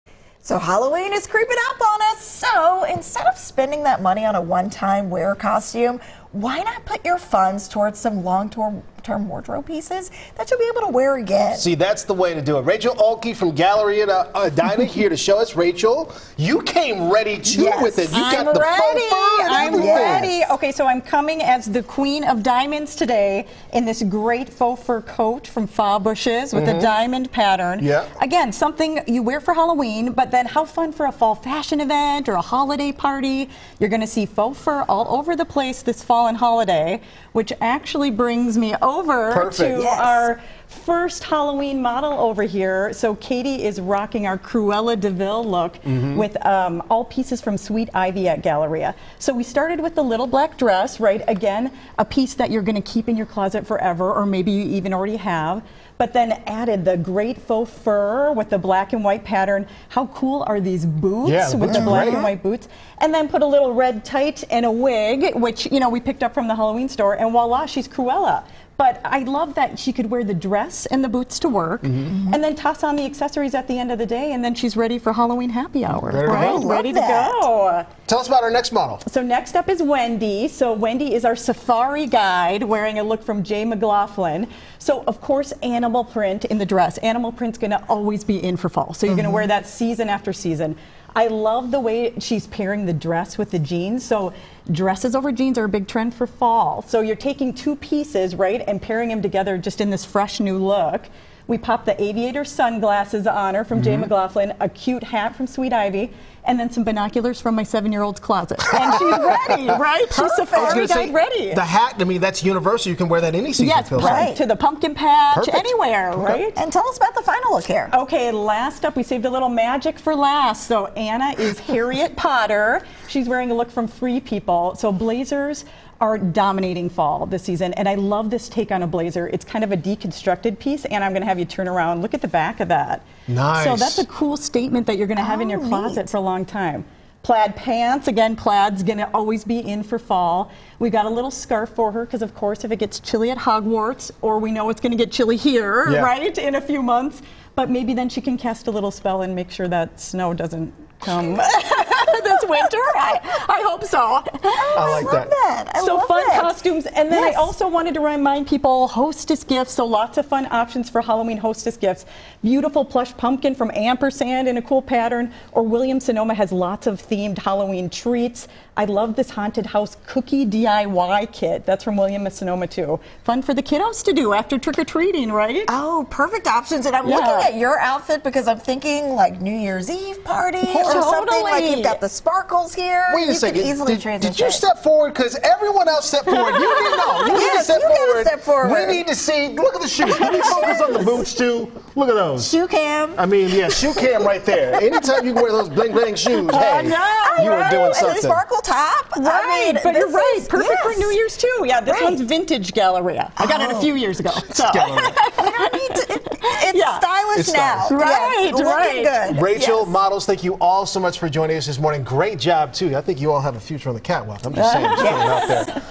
Give your Halloween costumes some longevity
Halloween is creeping up on us, so instead of spending on a one-time wear costume, why not put your funds toward long-term wardrobe pieces you’ll wear again? Rachel Oelke from Galleria Edina shows us how.